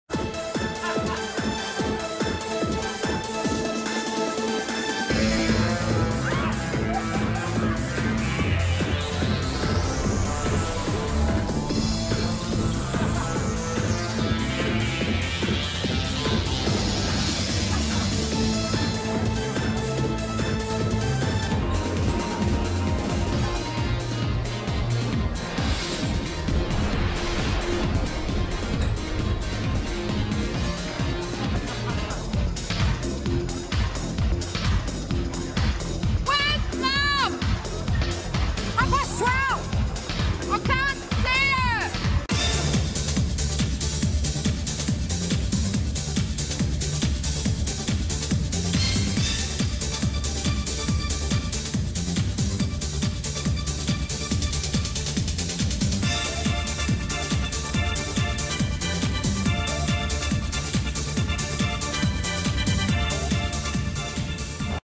Kath & Kim @ Metro Nightclub / Palace Theatre Melbourne
Two of Australia's hippest icons Kath & Kim tear up the dancefloor at Melbourne's world renowned Palace Theatre; former Metro Nightclub.